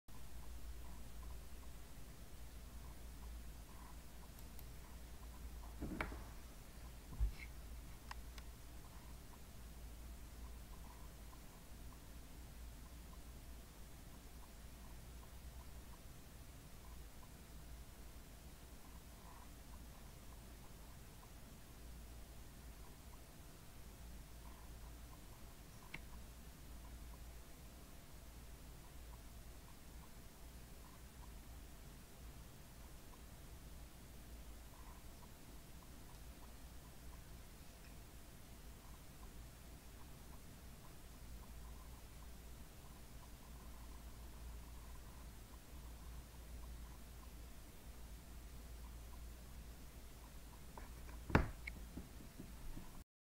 Men's Tommy Hilfiger Cool Sport Multi Function Watch 1791146
Black stainless steel case. White silicone strap. White dial. Multi-Function: Day, date and 24-hour subdials. Luminous hands. Case size: 46 mm. Case thickness: 12 mm. Band width: 22 mm. Scratch resistant: Mineral Crystal. Water resistant: 50 meters / 165 feet. UPC: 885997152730